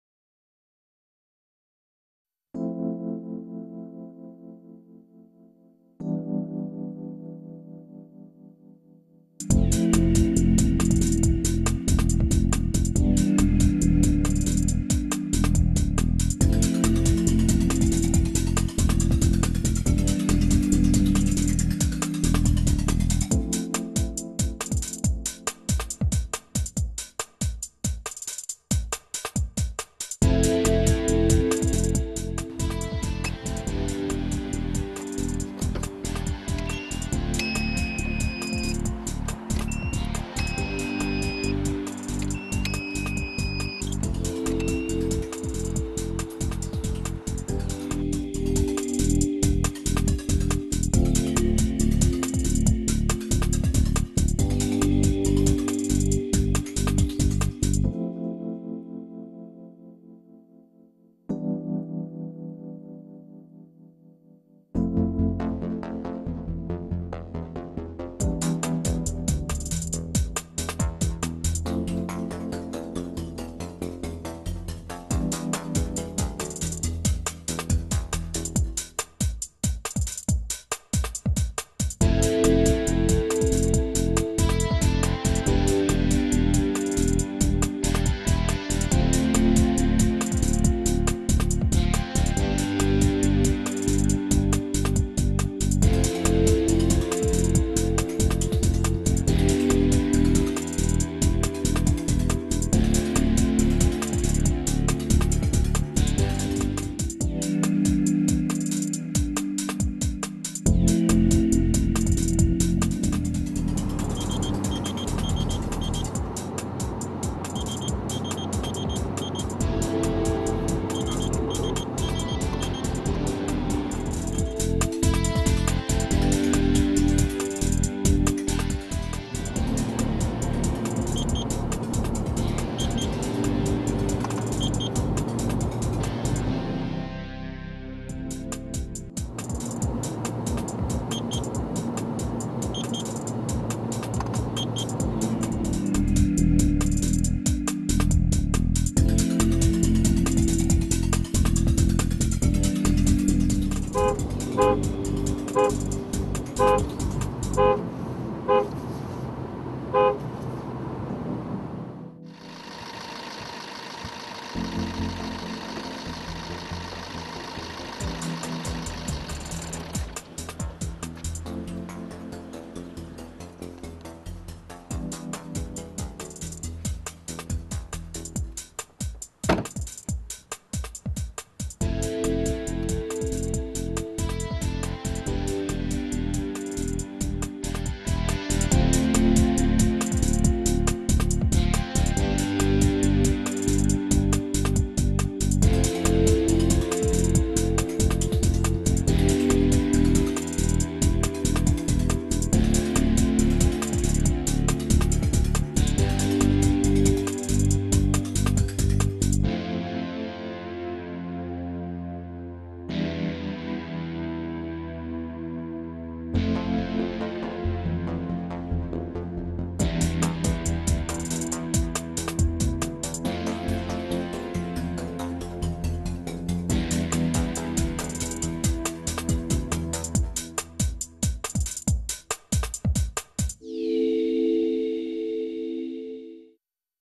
Presentazione Riflessometro per la Sicurezza Stradale
Dispositivo innovativo per rilevare l'insufficiente idoneità psicofisica alla guida conseguente all'assunzione di alcool, droghe, sostanze psicotrope, farmaci o a stanchezza.